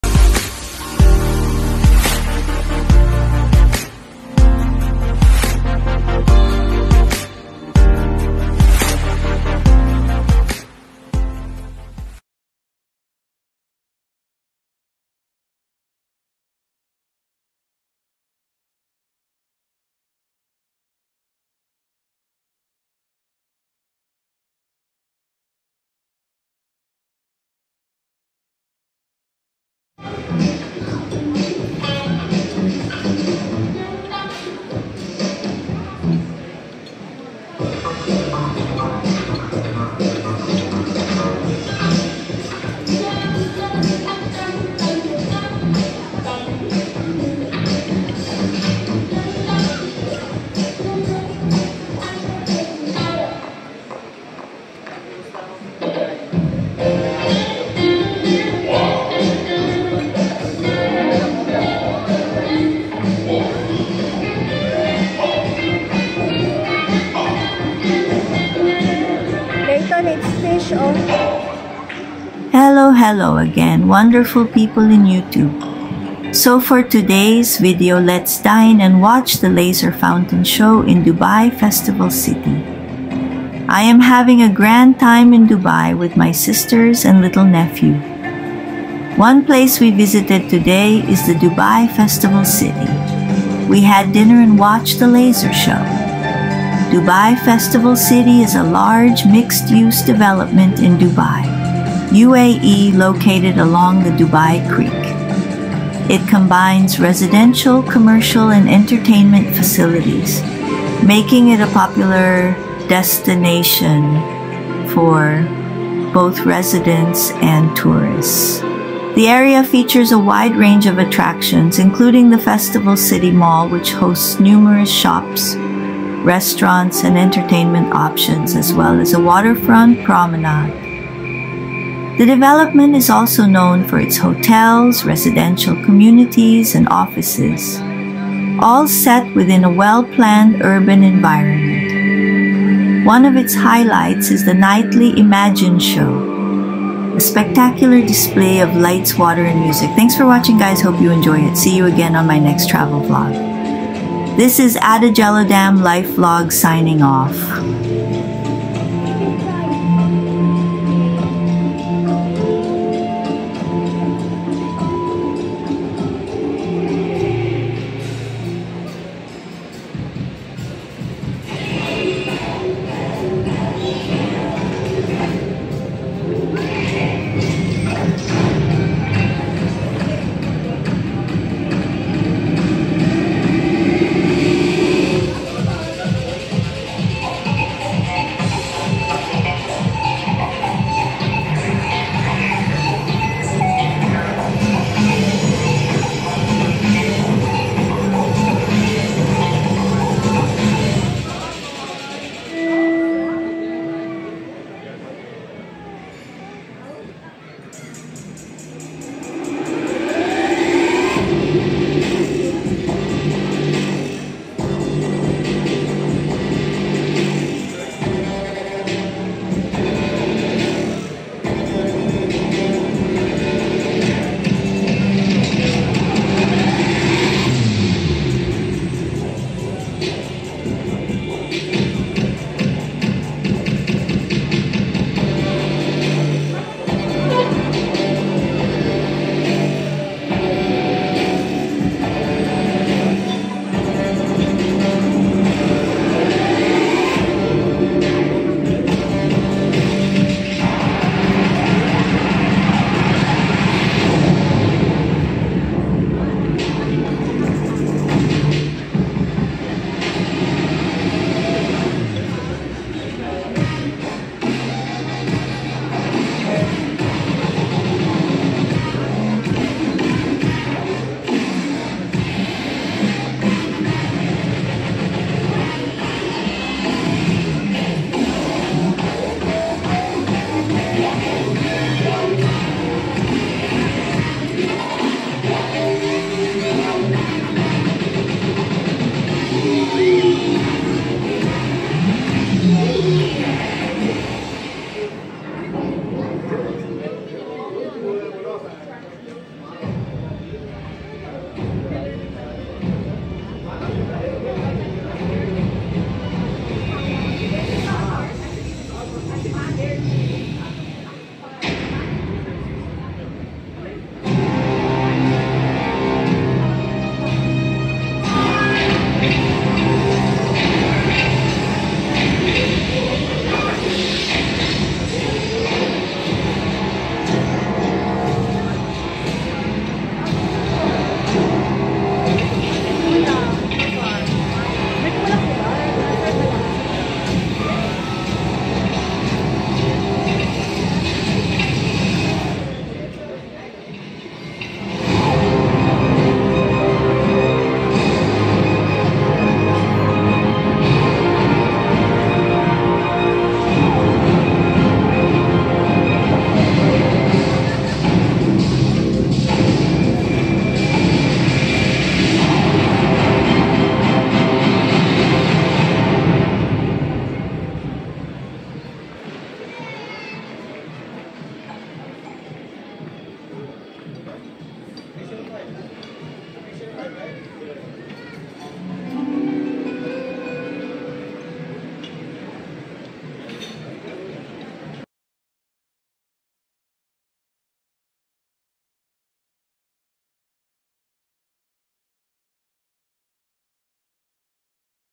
“Mesmerizing IMAGINE Show at Dubai Festival City: A Spectacle of Lights, Water & Music”
Dubai Festival City is a vibrant waterfront destination that blends luxury shopping, fine dining, and world-class entertainment. Home to the award-winning IMAGINE show, it offers an unforgettable sensory experience with stunning displays of light, water, and music that captivate audiences every night. Whether exploring the scenic promenade or witnessing the record-breaking show on Dubai Creek, Dubai Festival City is a must-visit for unforgettable moments in the city's heart.”